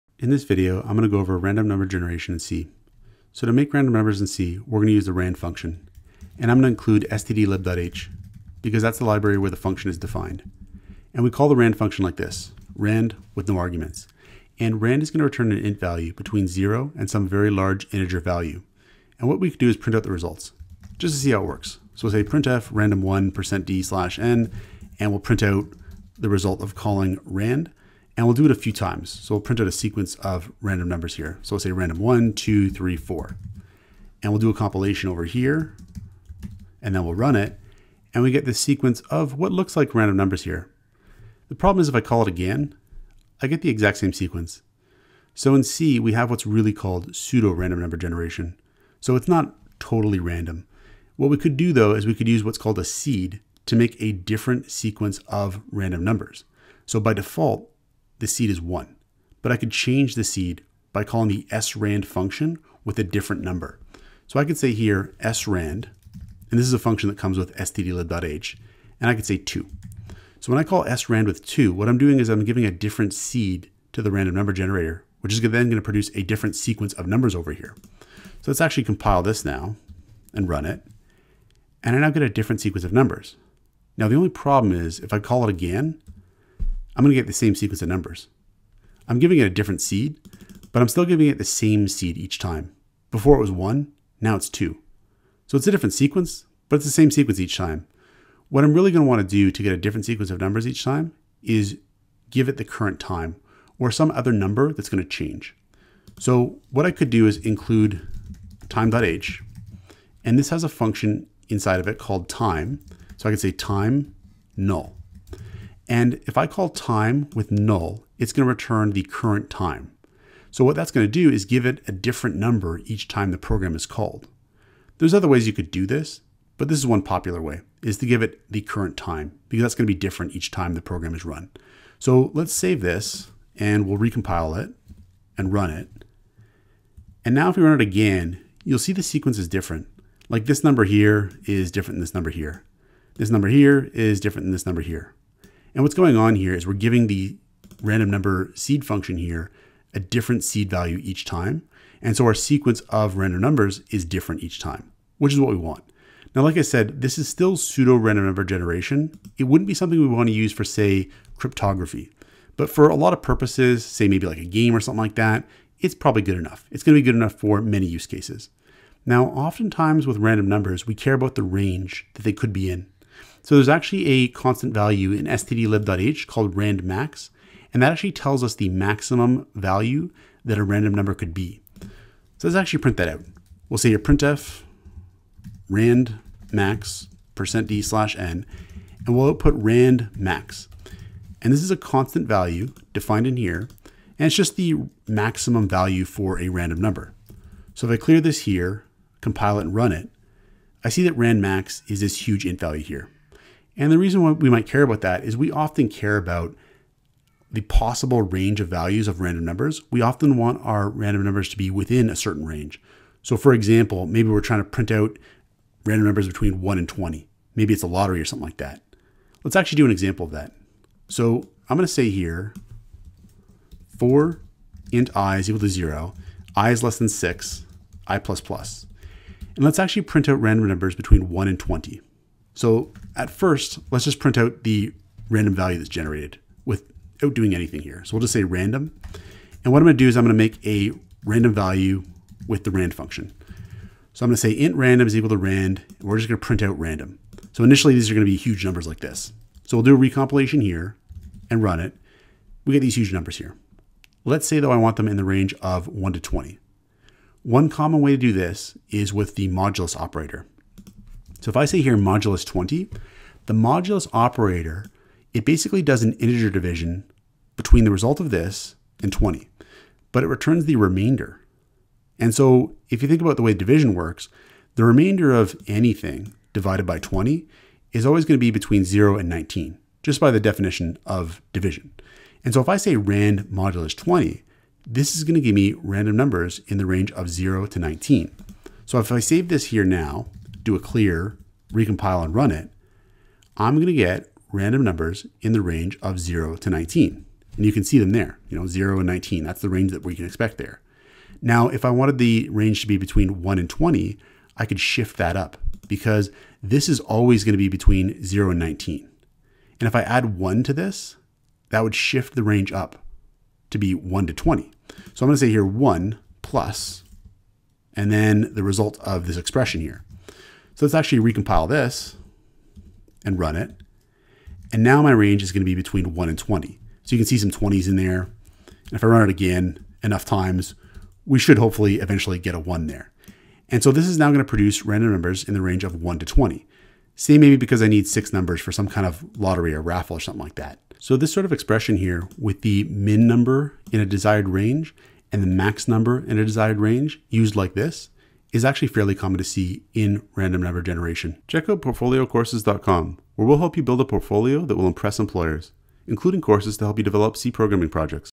Random Number Generation | C Programming Tutorial
An overview of how to do random number generation in C. Source code to build a portfolio that will impress employers!